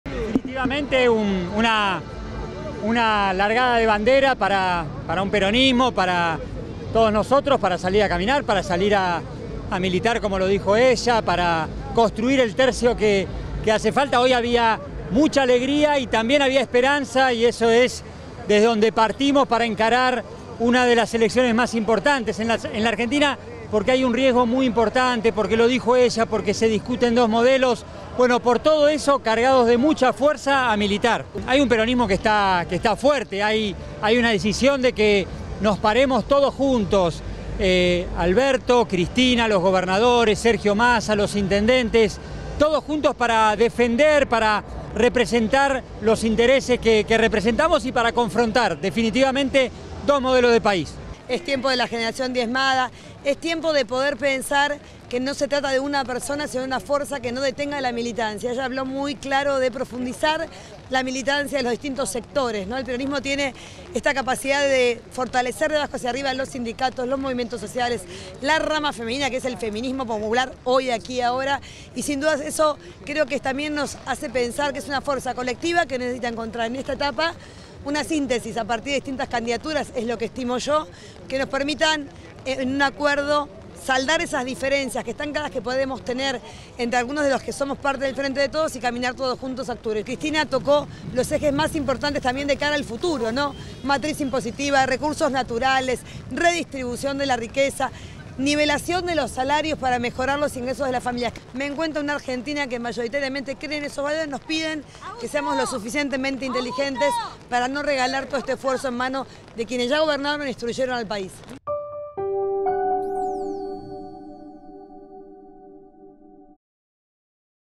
Katopodis: “Hay una decisión de pararnos todos juntos para confrontar dos modelos de país”
El ministro de Obras Públicas, Gabriel Katopodis, dijo tras el discurso de la vicepresidenta Cristina Fernández de Kirchner en Plaza de Mayo que “hay una decisión de pararnos todos juntos para confrontar dos modelos de país”.